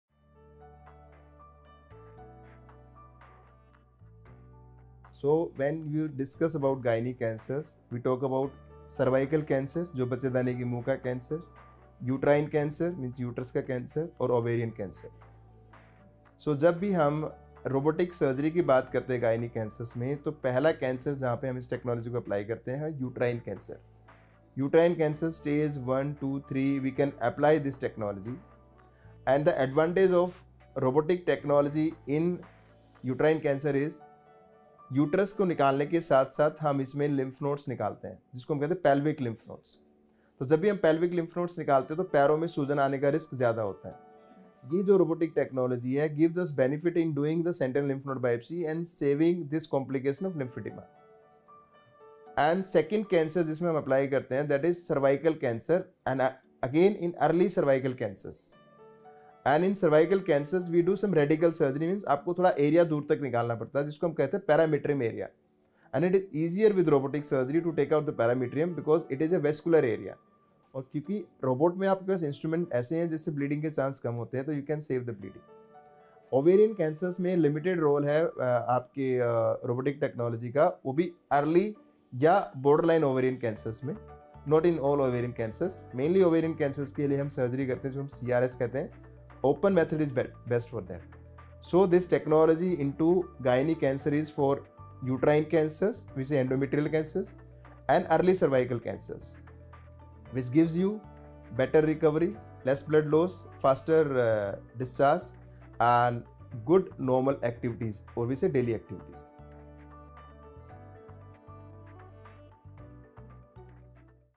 Role of Robotic Surgery in Gynae Cancer | Dr. Surender Kumar Dabas | BLK - Max Hospital
Robotic surgeries are mainly used for uterine or endometrial cancers and early cervical cancers among the three gynae cancers. Dr. Surender Kumar Dabas, Senior Director and HOD, Surgical Oncology and Robotic Surgery, BLK-Max Super Specialty Hospital, talks about the application of robotics in gynae cancer surgeries and its benefits which are - speedy recovery, reduced blood loss, and early discharge.